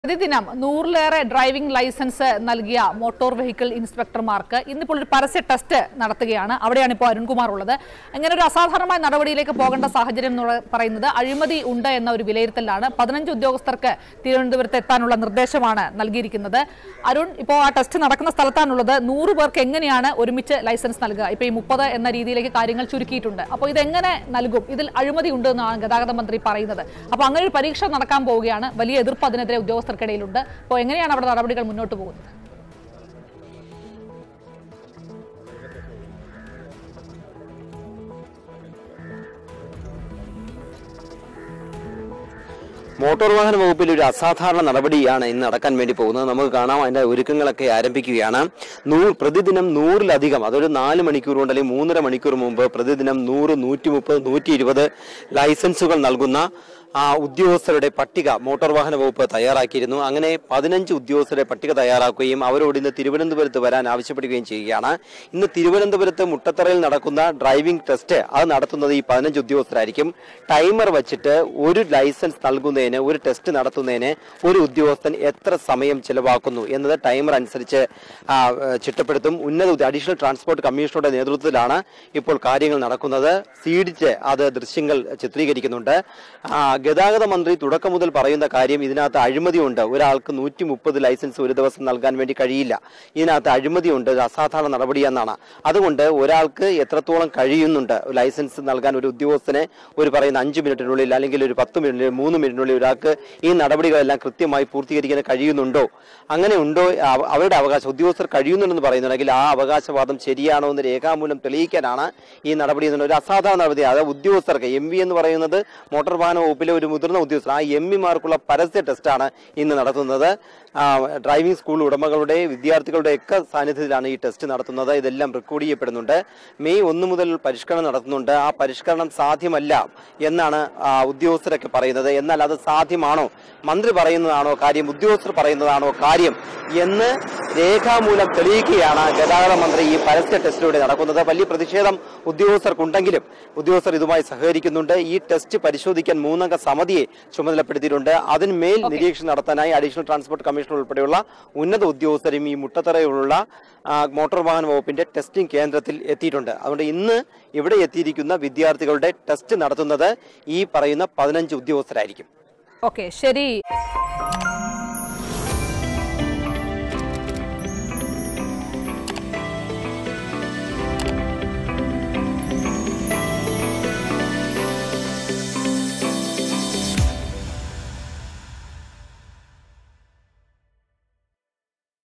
നൂറിലധികം ലൈസന്‍സ് കൊടുത്ത മോട്ടോര്‍ വെഹിക്കിള്‍ ഇന്‍സ്‌പെക്ടര്‍മാര്‍ക്ക് ഇന്ന് ടെസ്റ്റ്
പ്രതിദിനം നൂറിലധികം ഡ്രൈവിംഗ് ലൈസന്‍സ് നല്‍കിയ മോട്ടോര്‍ വെഹിക്കിള്‍ ഇന്‍സ്‌പെക്ടര്‍മാര്‍ക്ക് ഇന്ന് തിരുവനന്തപുരത്ത് പരസ്യ ടെസ്റ്റ്